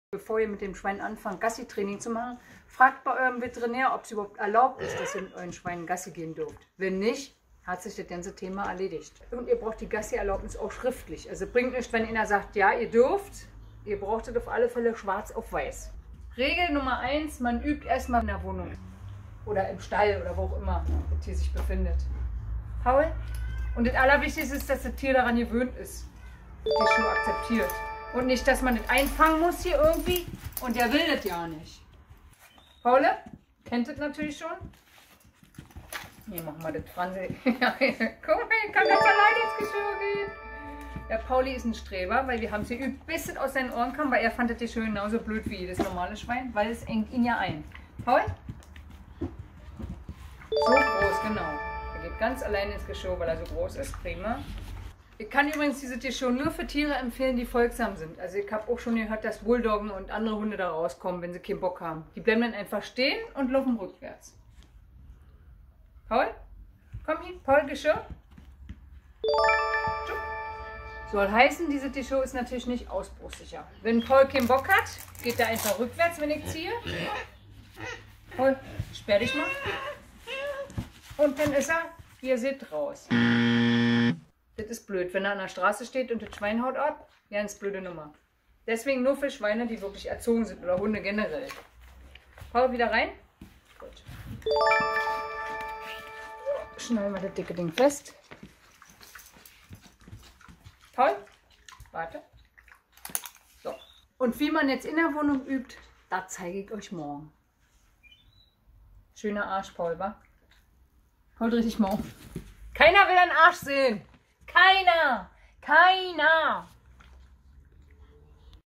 Wie man mit Minischweine Gassi geht 1: Geschirr-Training
Du hast ein Minischwein? Du möchtest ihm Abwechslung bieten? Du willst mit ihm Gassi gehen? Gefällt mir ! Aber bevor Du überhaupt daran denkst, melde Dich mal bei Deinem Veterinäramt und hole Dir eine schriftliche Genehmigung, das Du mit Deinen Schweinen überhaupt vom Hof runter darfst. Wenn das nicht erlaubt wird, dann sei fair und tue es auch nicht.

Du hast also die Genehmigung bekommen? In diesem Video zeige ich Dir, wie Du IN DER WOHNUNG mit dem Training beginnst, indem Du Deine Schweine an ein Geschirr gewöhnst.